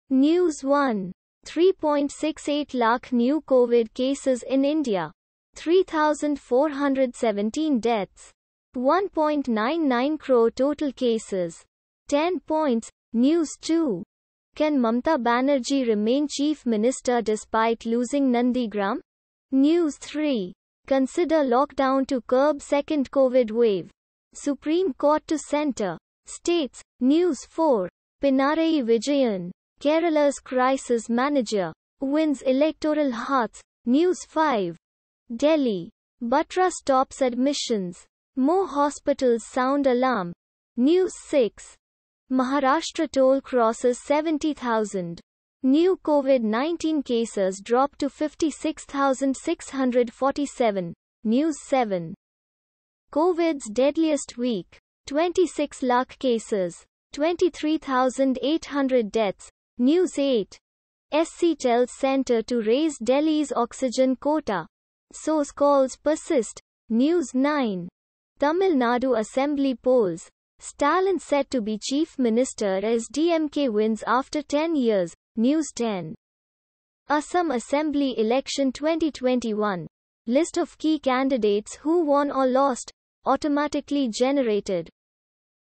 News Headlines (India)- 03/05/2021,11:00 AM
News 1:3.68 Lakh New Covid Cases In India, 3,417 Deaths; 1.99 Crore Total Cases: 10 Points, News 2:Can Mamata Banerjee remain chief minister despite losing Nandigram?, News 3:Consider Lockdown To Curb 2nd Covid Wave: Supreme Court To Centre, States, News 4:Pinarayi Vijayan, Kerala’s Crisis Manager, Wins Electoral Hearts, News 5:Delhi: Batra stops admissions, more hospitals sound alarm, News 6:Maharashtra toll crosses 70,000; new Covid-19 cases drop to 56,647, News 7:Covid’s deadliest week: 26 lakh cases, 23,800 deaths, News 8:SC tells Centre to raise Delhi’s oxygen quota; SOS calls persist, News 9:Tamil Nadu Assembly polls | Stalin set to be Chief Minister as DMK wins after 10 years, News 10:Assam Assembly Election 2021 | List Of Key Candidates Who Won Or Lost. Automatically Generated For Advertisement Contact